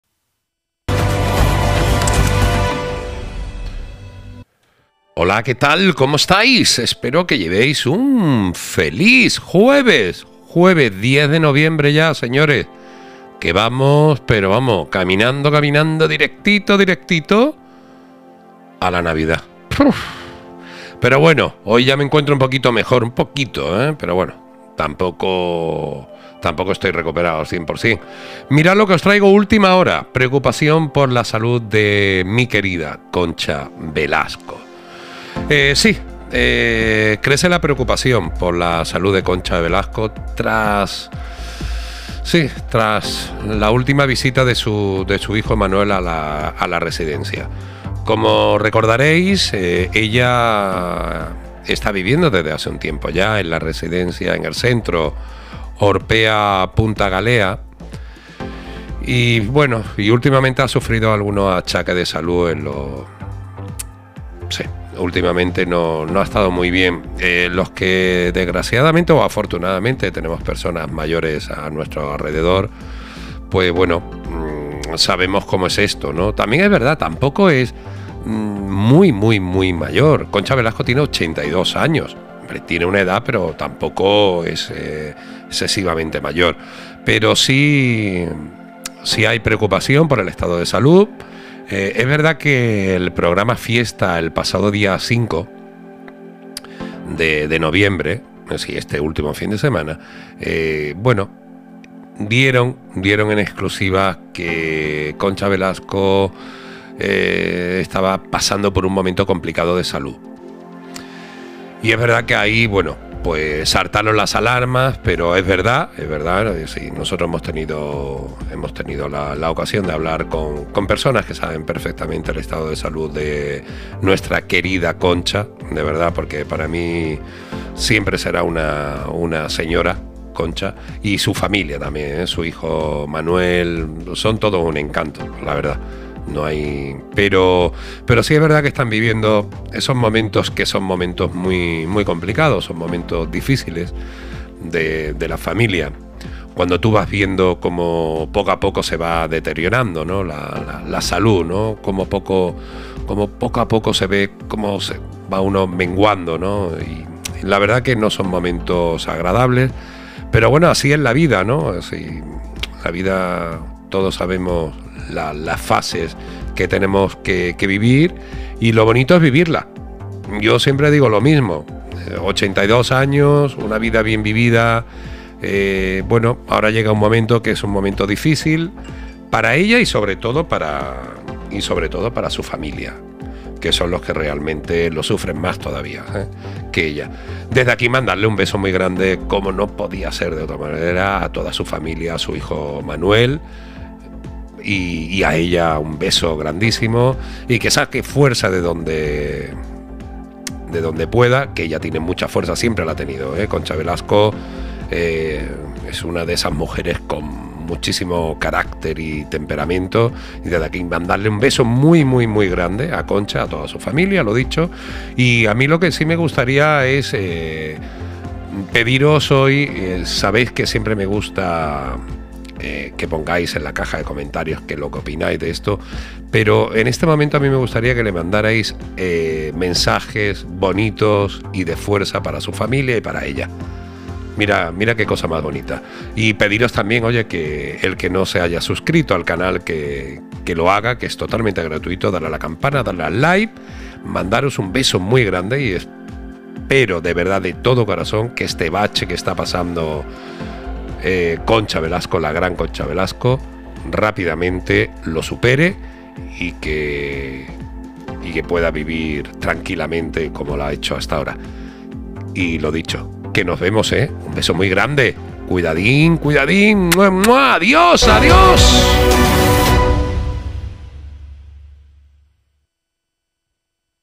Preocupación por la salud de Concha Velasco.
La veterana actriz, que vive en el exclusivo centro Orpea Punta Galea, ha sufrido algunos achaques de salud en los últimos meses.